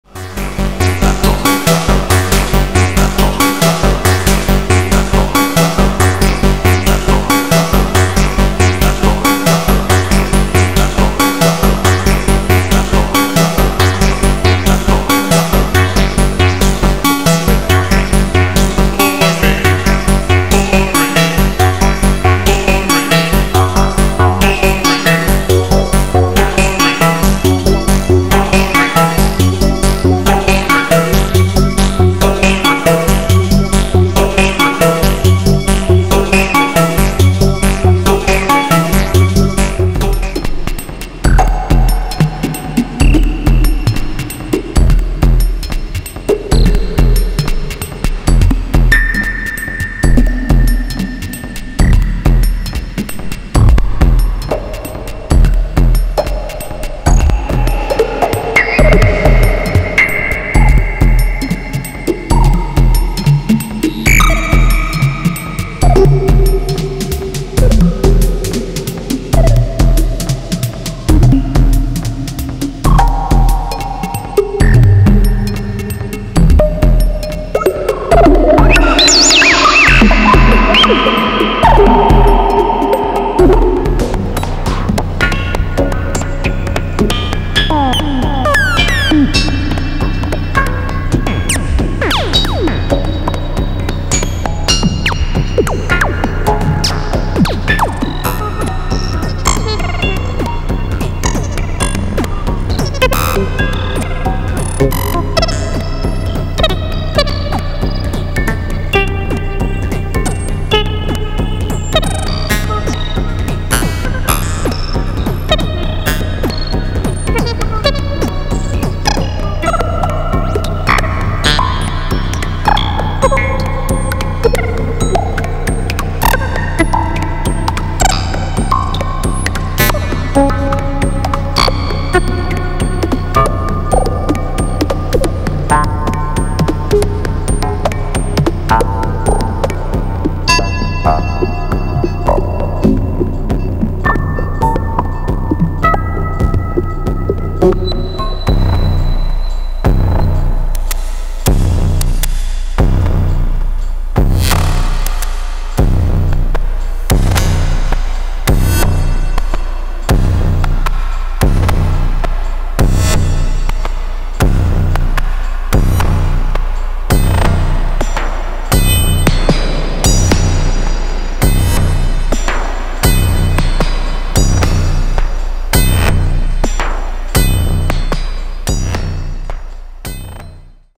10.25.2024 Mos-lab 2500 & Scrotum Lab [Excerpts]
Exploring diverse sonic territories with the Mos-lab 2500 and a couple of Scrotum Lab modules (Radio Music & Orgone Accumulator MK1).